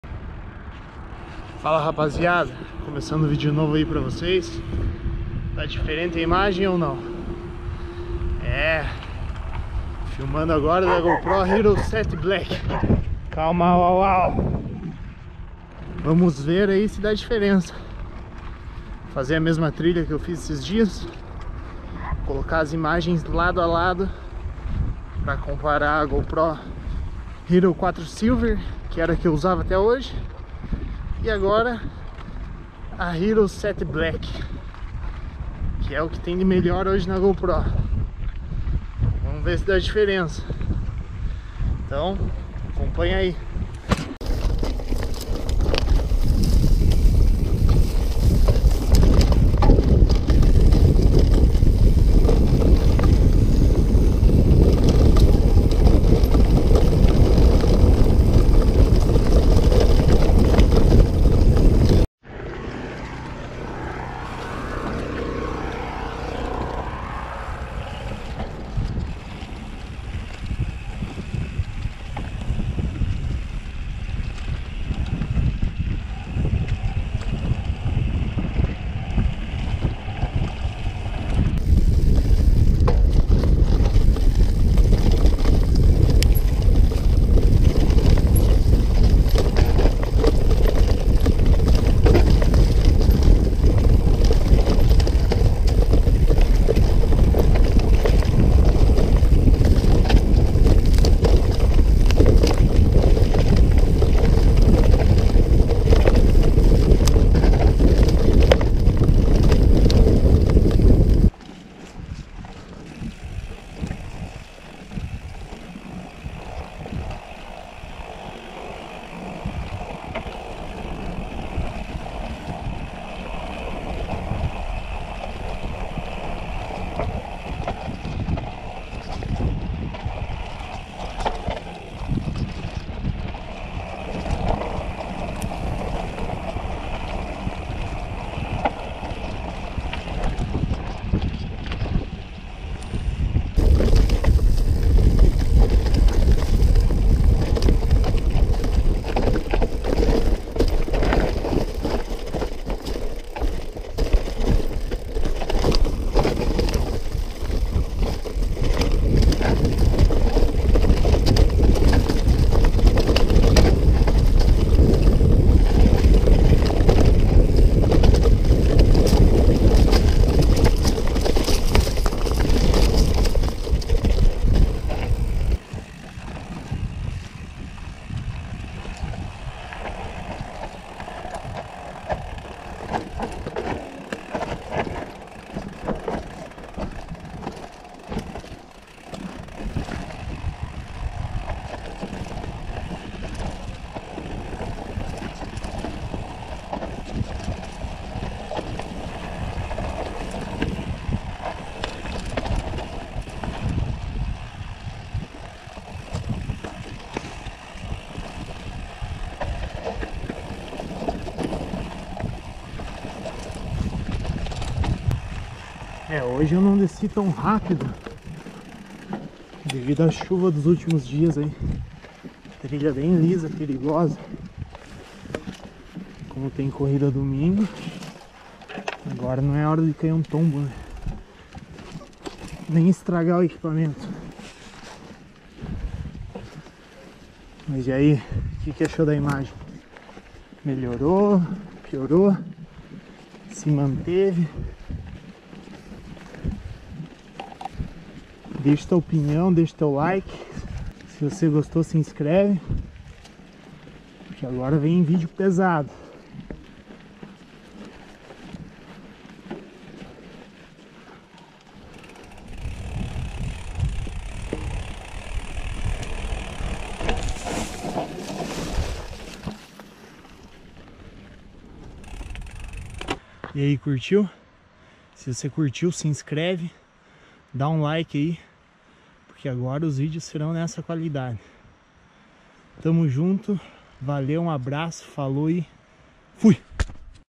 COMPARANDO A GOPRO 4 COM A GOPRO 7
Galera, esse foi um primeiro teste com a minha nova Gopro, comparei a imagem da nova com a da antiga numa trilha com bastante tripidação, acho que o salto de qualidade da 4 pra 7 foi alto. E vc o que achou?